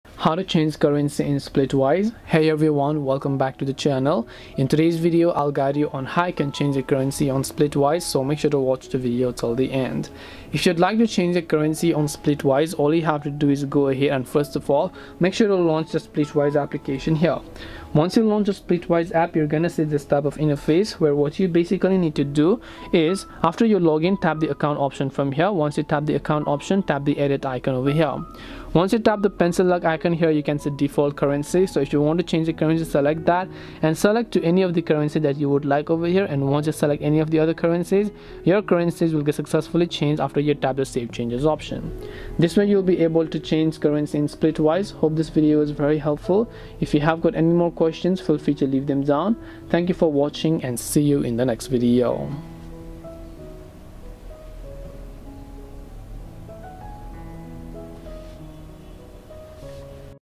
How to Change Currency in SplitWise !
In this tutorial video, I will quickly guide you on how you can change currency in Splitwise. So, make sure to watch this video till the end. We try to be responsive to as many questions as possible.

► Chapters:
0:00 Introduction
0:10 Change Currency
0:45 Conclusion